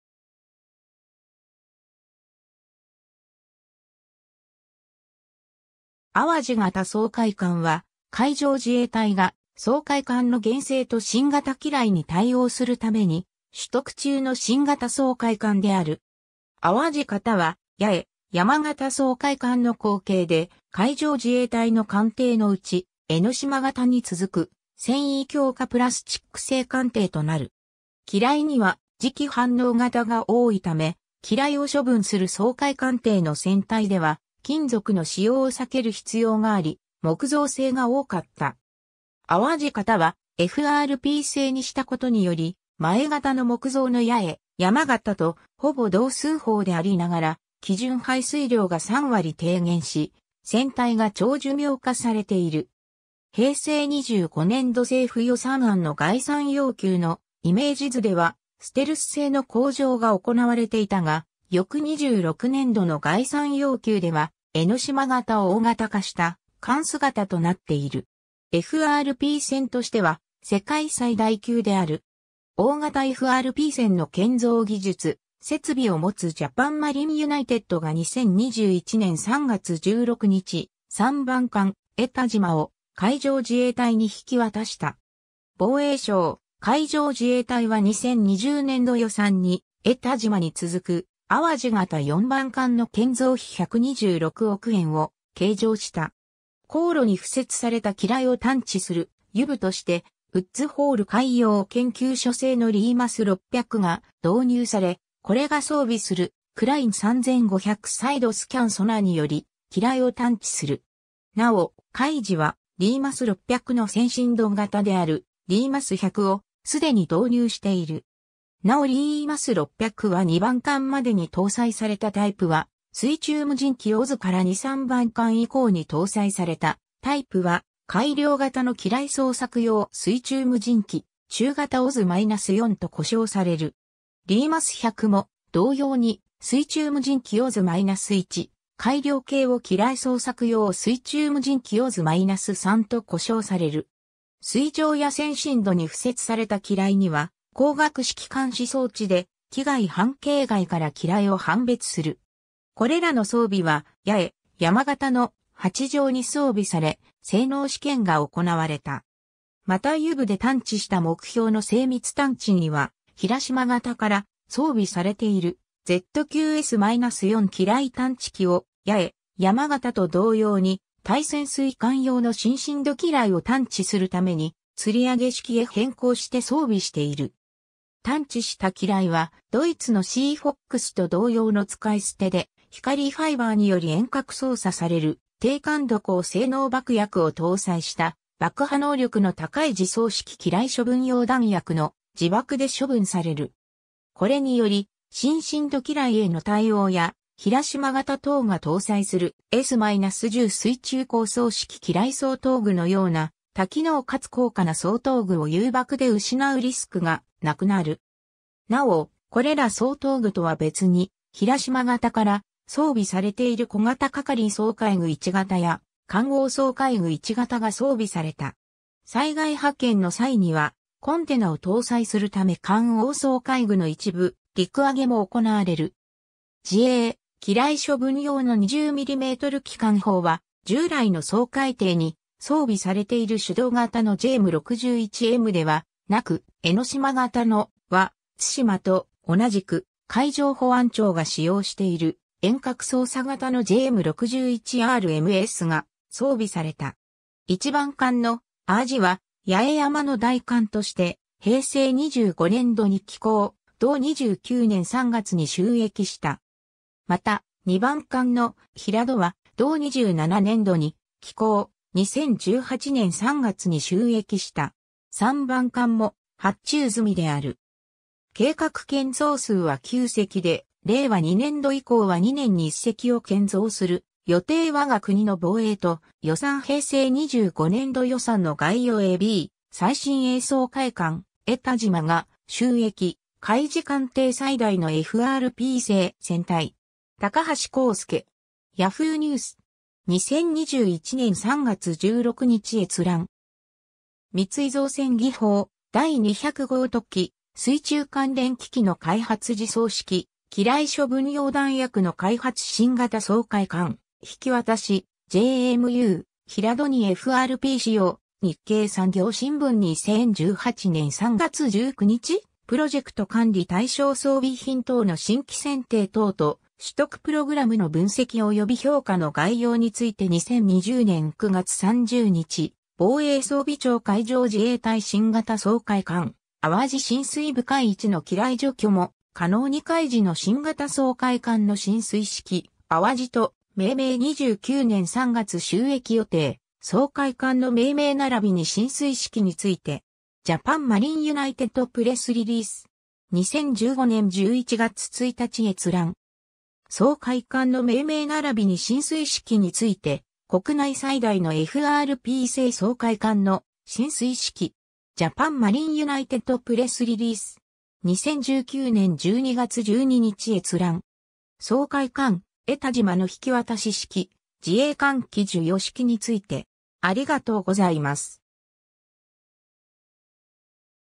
あわじ型掃海艦
あわじ型掃海艦（あわじがた そうかいかん、英語: Awaji-class minesweeper）は海上自衛隊が掃海艦の減勢と新型機雷に対応するために取得中の新型掃海艦である。
 あわじ型は、やえやま型掃海艦の後継で、海上自衛隊の艦艇のうち、えのしま型に続く繊維強化プラスチック（FRP）製艦艇となる。
機雷には磁気反応型が多いため、機雷を処分する掃海艦艇の船体では金属の使用を避ける必要があり、木造製が多かった。
あわじ型はFRP製にしたことにより、前型の木造のやえやま型とほぼ同寸法でありながら、基準排水量が3割低減し、船体が長寿命化されている。
平成25年度（2013年度）政府予算案の概算要求のイメージ図ではステルス性の向上が行われていたが、翌26年度（2014年度）の概算要求では、えのしま型を大型化した艦容となっている。
 FRP船としては世界最大級である。
大型FRP船の建造技術・設備を持つジャパンマリンユナイテッド（JMU）が2021年3月16日、3番艦「えたじま」を海上自衛隊に引き渡した。
防衛省・海上自衛隊は2020年度予算に、えたじまに続くあわじ型４番艦の建造費126億円を計上した。
 航路に敷設された機雷を探知するUUVとしてウッズホール海洋研究所製のリーマス600が導入され、これが装備するクライン3500サイドスキャン・ソナーにより機雷を探知する。
なお、海自はリーマス600の浅深度型であるリーマス100を既に導入している。
なおリーマス600は2番艦までに搭載されたタイプは水中無人機OZZ-2、3番艦以降に搭載されたタイプは改良型の機雷捜索用水中無人機(中型）OZZ-4と呼称される。
リーマス100も同様に水中無人機OZZ-1、改良型を機雷捜索用水中無人機(小型)OZZ-3と呼称される。
 水上や浅深度に敷設された機雷には光学式監視装置（レーザー・レーダー）で危害半径外から機雷を判別する。
これらの装備は、やえやま型の「はちじょう」に装備され、性能試験が行われた。
 またUUVで探知した目標の精密探知には、ひらしま型から装備されているZQS-4機雷探知機を、やえやま型と同様に対潜水艦用の深深度機雷を探知するために、吊り上げ式（VDS）へ変更して装備している。
 探知した機雷はドイツのシーフォックスと同様の使い捨てで、光ファイバーにより遠隔操作される低感度高性能爆薬を搭載した爆破能力の高い自走式機雷処分用弾薬（EMD）の自爆で処分される（三井造船開発・製造）。
これにより深深度機雷への対応や、ひらしま型等が搭載するS-10水中航走式機雷掃討具のような多機能かつ高価な掃討具を誘爆で失うリスクが無くなる。
 なお、これら掃討具とは別に、ひらしま型から装備されている小型係維掃海具1型や感応掃海具1型が装備された。
災害派遣の際には、コンテナを搭載するため感応掃海具の一部陸揚げも行われる。
 自衛・機雷処分用の20mm機関砲は従来の掃海艇に装備されている手動型のJM61-Mではなく、えのしま型の「はつしま」と同じく、海上保安庁が使用している遠隔操作型のJM61R-MSが装備された。
 1番艦の「あわじ」（25MSO、183億円）は「やえやま」の代艦として平成25年度（2013年度）に起工、同29年（2017年）3月に就役した。
また、2番艦の「ひらど」（26MSO、186億円）は同27年度（2015年度）に起工、2018年3月に就役した。
3番艦も発注済みである。
 計画建造数は9隻で、令和2年度以降は2年に1隻を建造する予定 ^ 我が国の防衛と予算 平成25年度予算の概要 (PDF) ^ a b “最新鋭掃海艦「えたじま」が就役――海自艦艇最大のFRP製船体”. 高橋浩祐. Yahoo!ニュース (2021年3月16日). 2021年3月16日閲覧。
 ^ 三井造船技報　第200号 特機・水中関連機器の開発 (PDF) 、自走式機雷処分用弾薬の開発 (PDF) ^ 新型掃海艦引き渡し／JMU「ひらど」にFRP使用『日経産業新聞』2018年3月19日（自動車・機械面） ^ プロジェクト管理対象装備品等の新規選定等と取得プログラムの分析及び評価の概要について2020年9月30日、防衛装備庁 ^ 海上自衛隊 新型掃海艦「あわじ」進水 深い位置の機雷除去も可能に ^ 海自の新型掃海艦の進水式　「あわじ」と命名　２９年３月就役予定 ^ “掃海艦の命名ならびに進水式について”. ジャパン マリンユナイテッドプレスリリース (20...